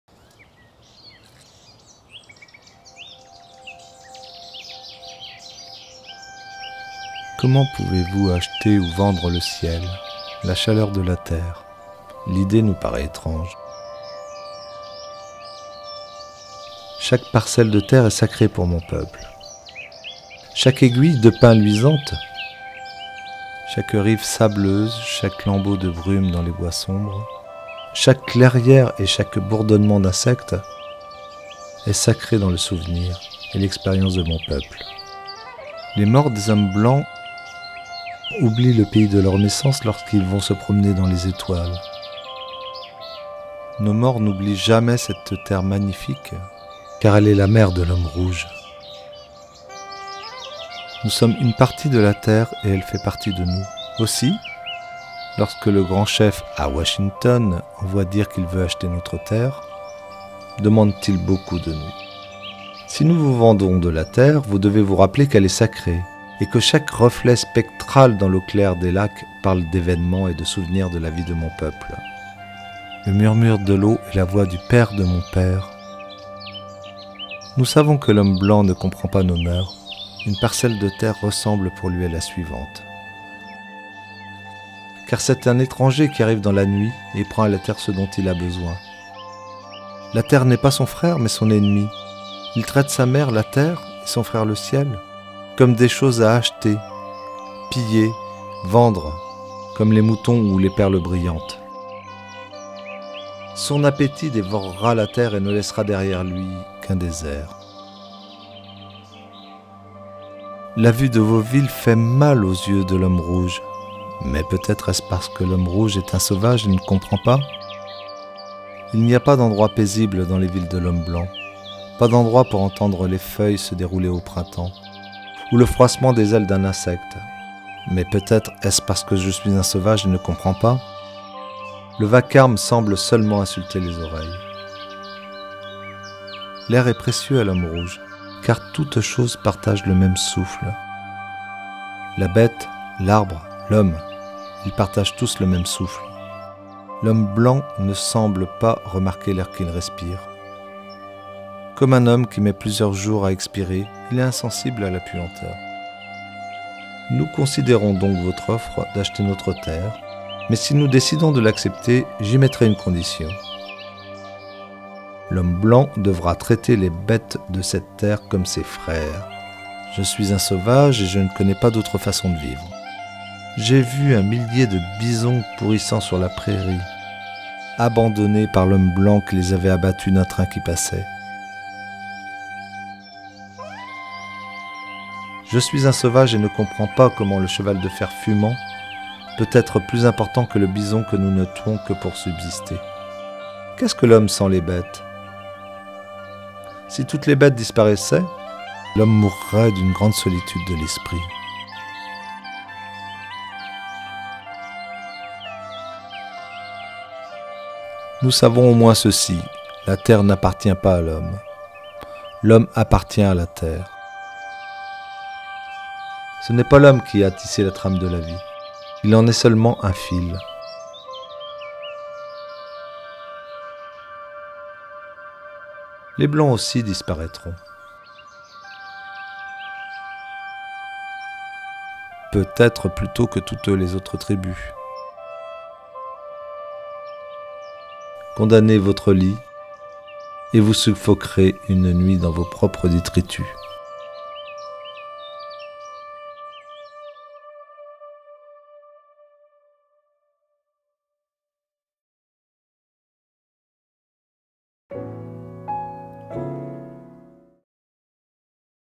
zad vue du ciel
La Zad vue du ciel en juillet 2017. Discours du chef Seattle prononcé à Washington en 1854. Réal. Christian Baudu